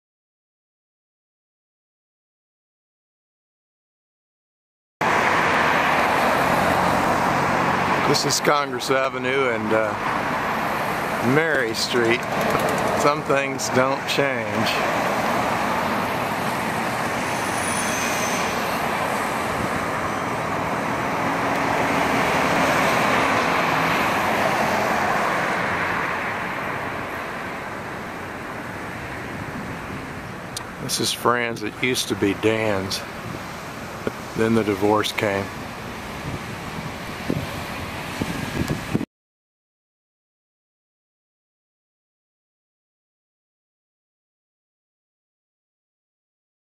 Austin Today Congress ave at Mary st.avi
A landmark section of S. Congress Avenue in Austin, Texas, October 2011.